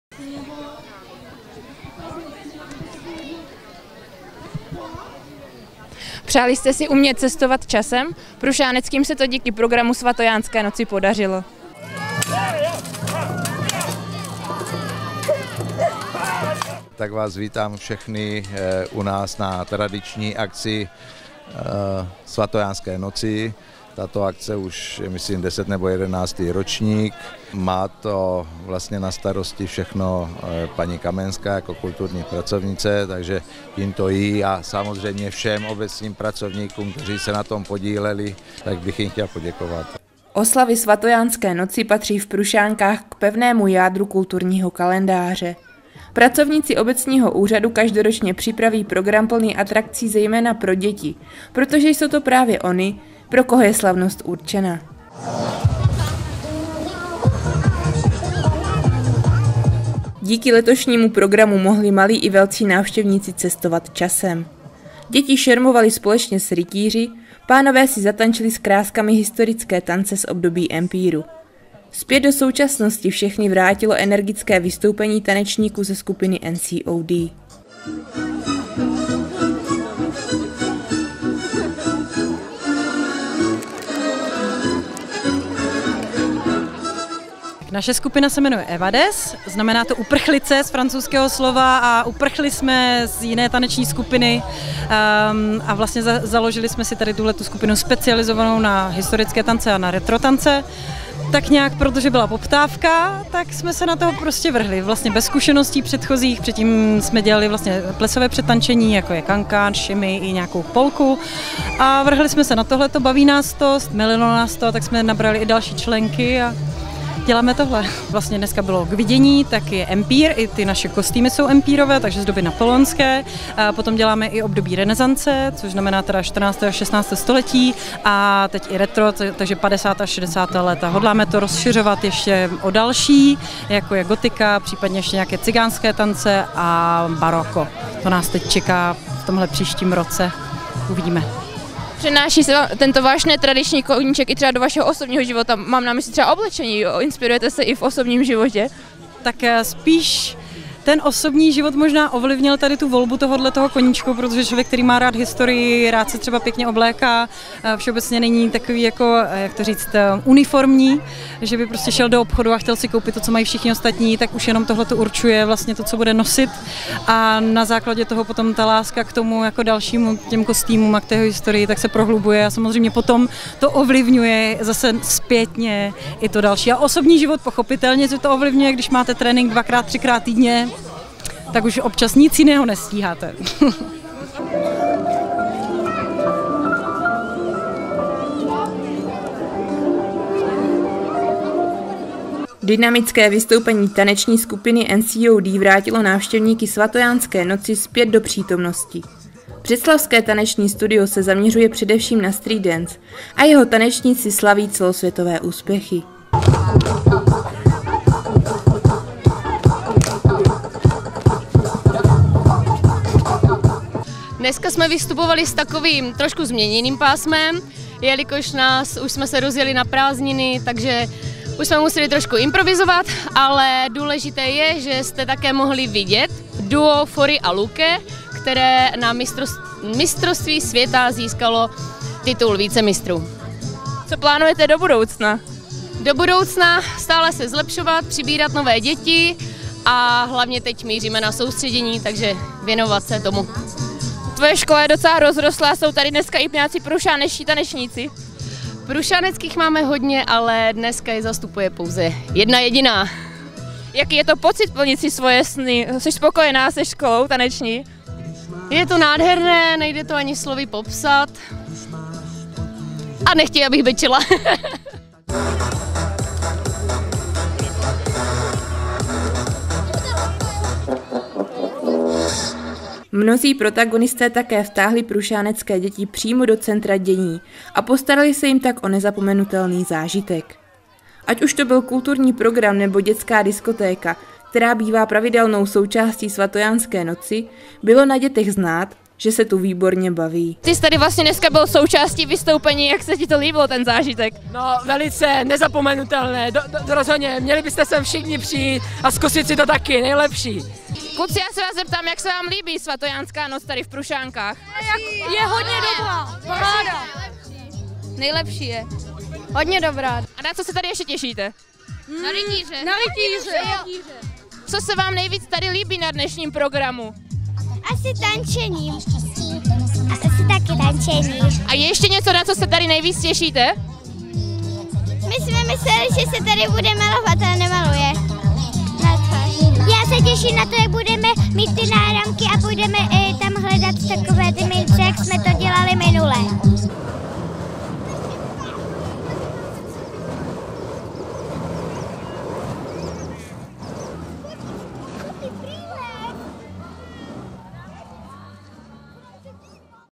Svatojánská noc přenesla prušánecké do dob dávno minulých.
Oslavy svatojánské noci patří v Prušánkách k pevnému jádru kulturního kalendáře. Pracovníci obecního úřadu každoročně připraví program plný atrakcí zejména pro děti, protože jsou to právě ony, pro koho je slavnost určena.